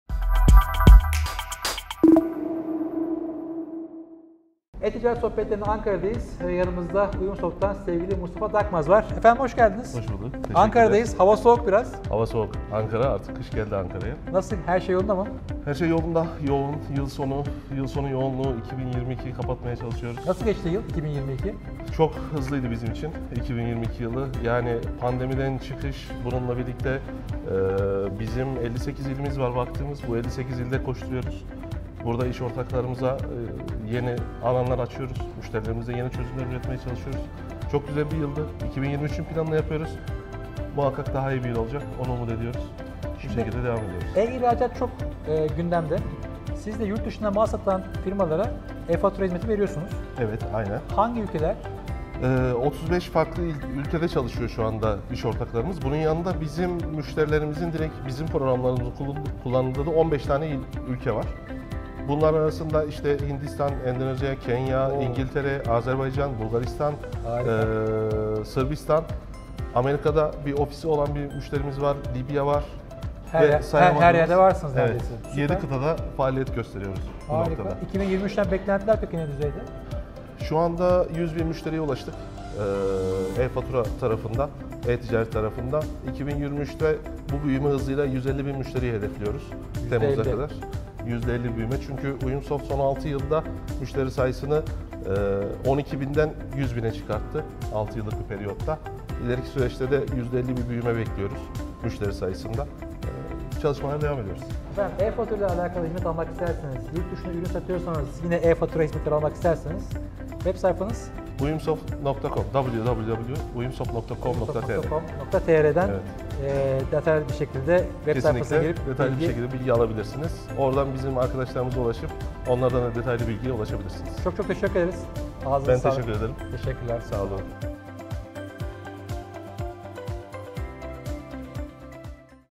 Uyumsoft E-Fatura Kanalını Büyütüyor. Mustafa Dakmaz ile Ankara'da Konuştuk.
Ankara'da düzenlediğimiz E-Ticaret Sohbetleri Anadolu adlı program öncesinde Uyumsoft ekoTicari Satış Takım Lideri Mustafa Dakmaz ile yaptığımız röportaja buradan bakabilirsiniz.